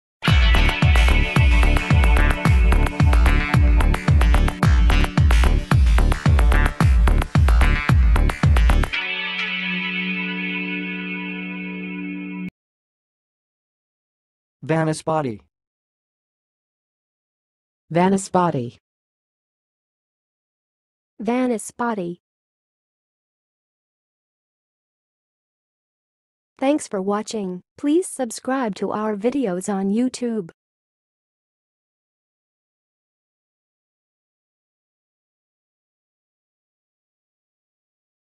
How to say "vanaspati"! (High Quality Voices)
Watch in this video how to say and pronounce "vanaspati"!